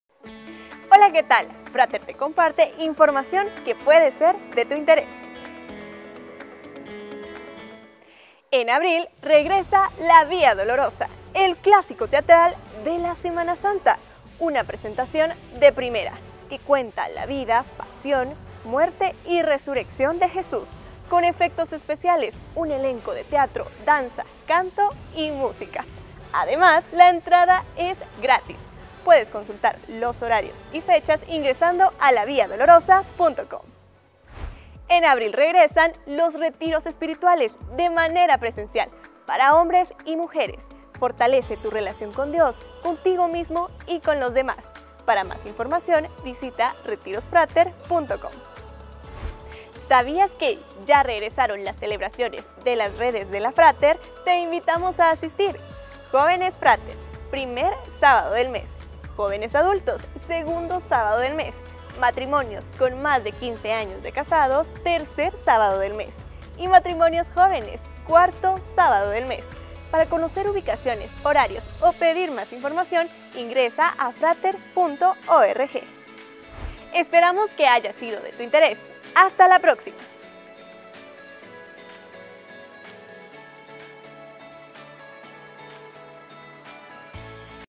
Eventos que no te puedes perder. 🔥
Se parte de La Vía Dolorosa, retiros espirituales y próximas celebraciones. Mantengámonos firmes, activos, comprometidos y sigamos siendo transformados.

¡Te esperamos en La Fráter, una iglesia cristiana para la familia! 🙌🏼